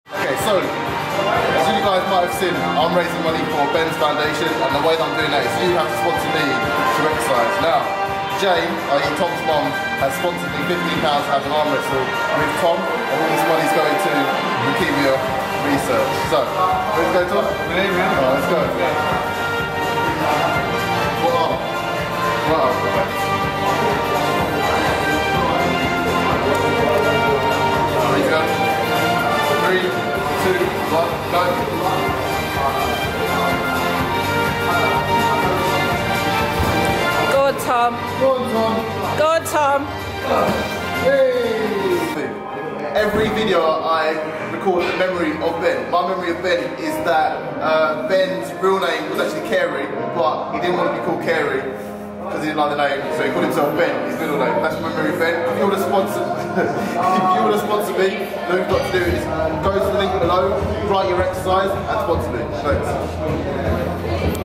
I Challenge Power Lifter, Tom Hawkins to an Arm Wrestle to raise money for Bens Foundation
I'm raising money for Bens Foundation. A charity in memory of my friend, Ben Johnson who passed unexpectedly due to Leukaemia, 7 years ago age 17.

I'm doing this to raise money for Bens Foundation until 16th July when I will be completing the 3 Peak Challenge to raise even more money